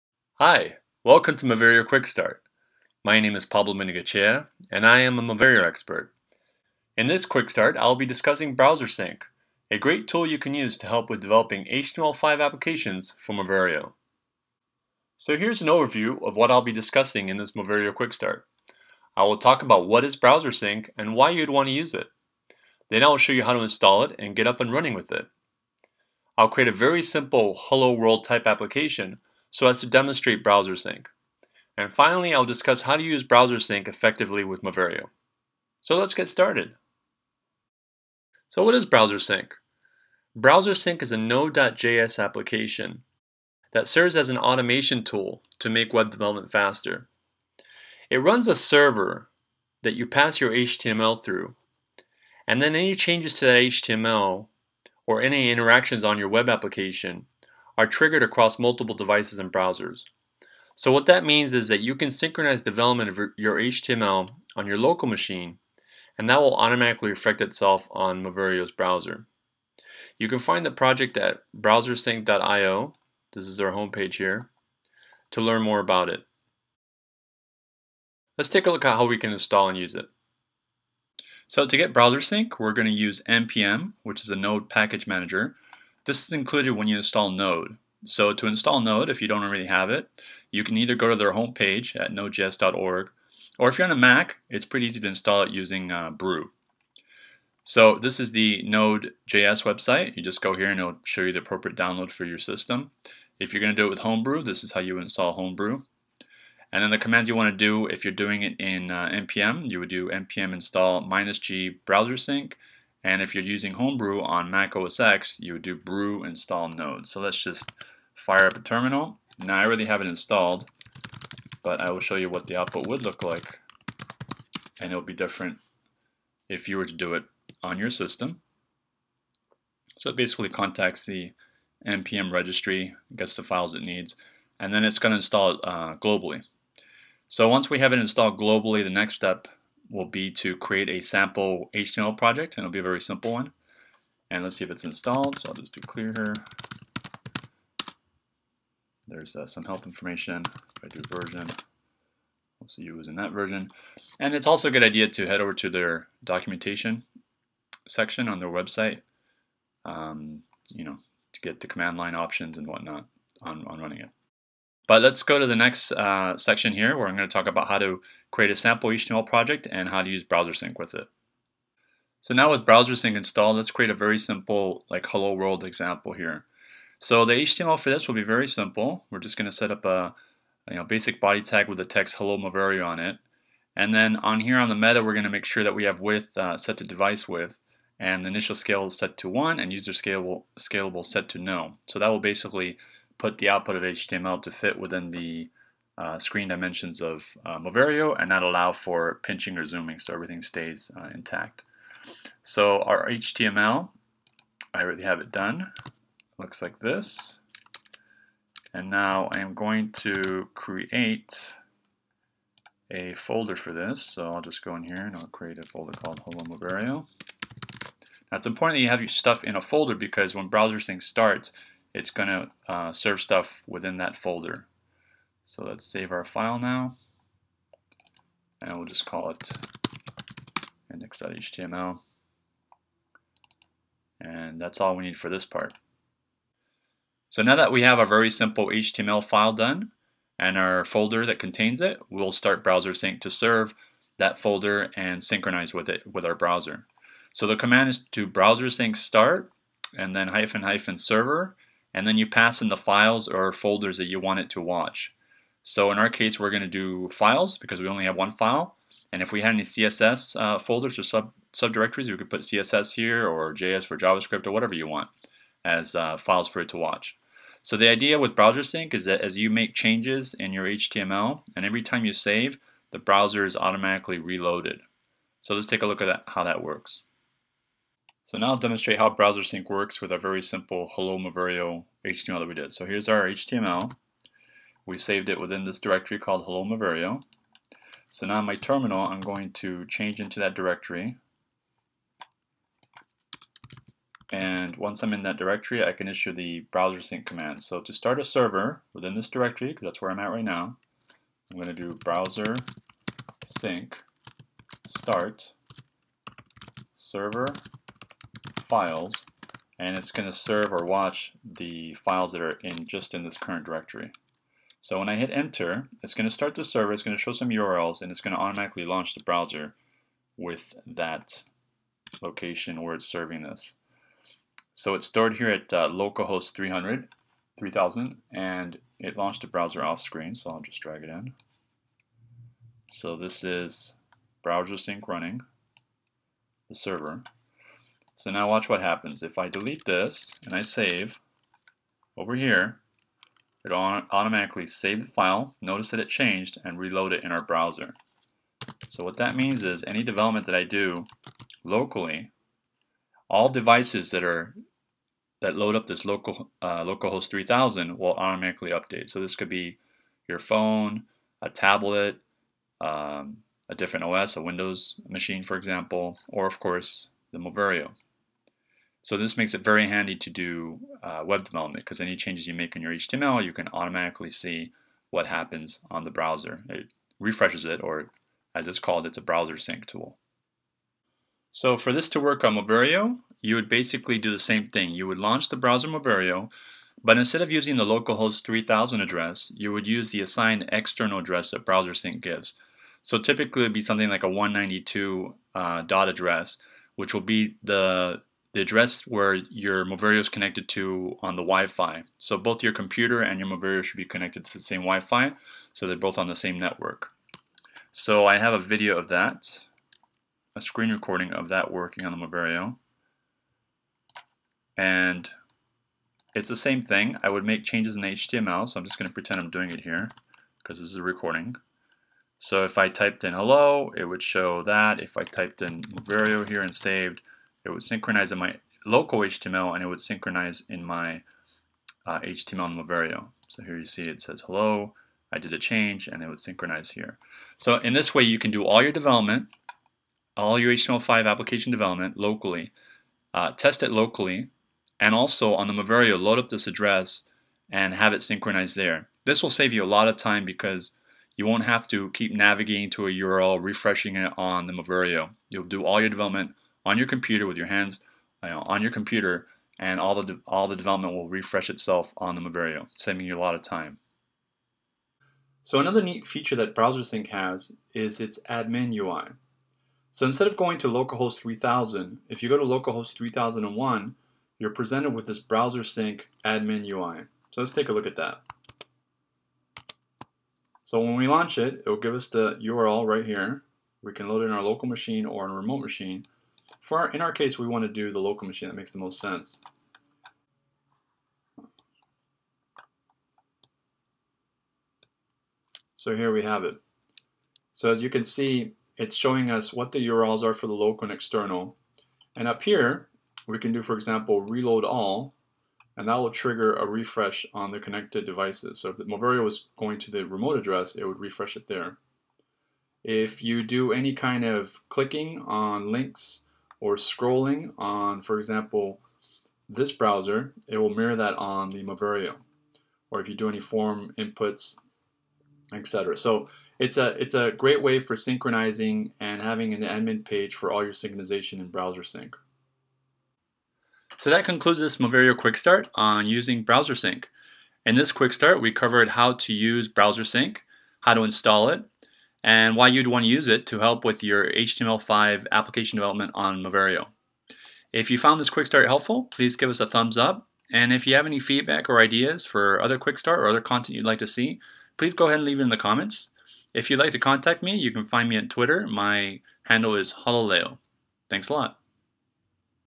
Moverio BT-300 - Quickstart: Using Browsersync for HTML5 App Development
Learn how to use Browsersync to develop HTML5 Moverio apps.

Agenda:
- What is Browsersync?
- Installing Browsersync
- Creating a simple 'Hello Moverio' project
- Browsersync demo
- Using Browsersync with Moverio